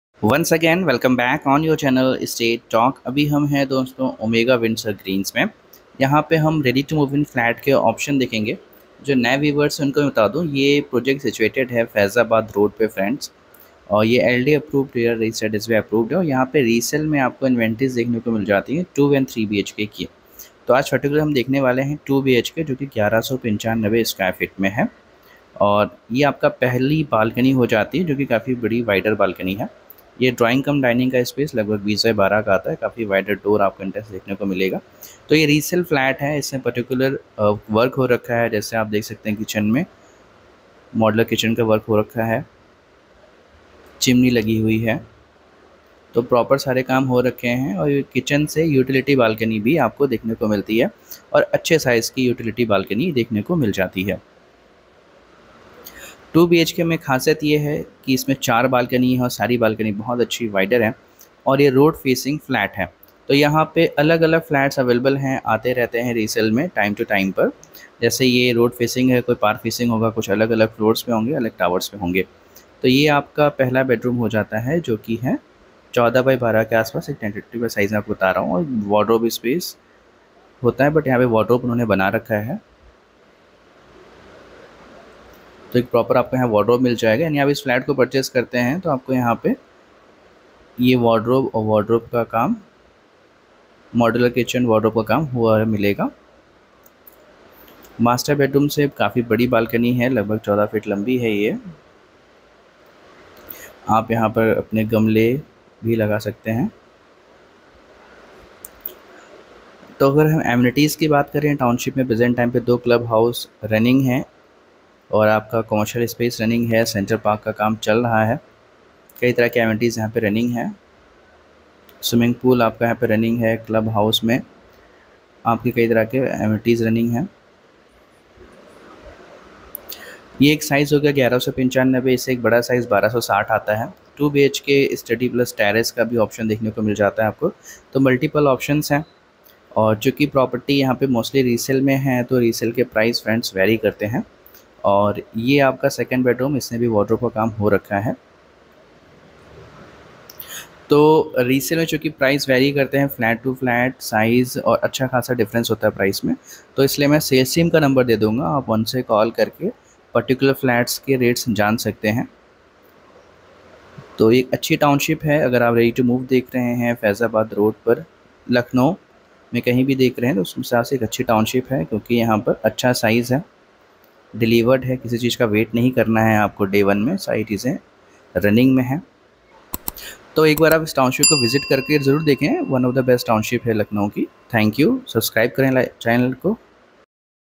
Ready to move flat in Resell| Omega Windsor Greens| Faizabad Road, Lucknow| Call:6393545456
QUERY- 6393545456 / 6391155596 ( CALL / WHATSAPP)

Omega Windsor Greens Occupying 7.50 acres on the burgeoning Faizabad area comprises 10 towers and scaling up to Basement + G + 14 floors. Windsor Greens offers 2BHk & 3BHK exclusive homes. Add to it the top-class amenities the apartment comes loaded with and you have a home that lets you enjoy quality time with your family and also some me-time with yourself

Rera Registration:-
PHASE-1:UPRERAPRJ17311
PHASE-2:UPRERAPRJ919772
PHASE-3:UPRERAPRJ848407
Phase-4:  UPRERAPRJ831593

CONSULTANTNAME:HARI PRASAD
CONSULTANT RERA NO:UPRERAAGT21851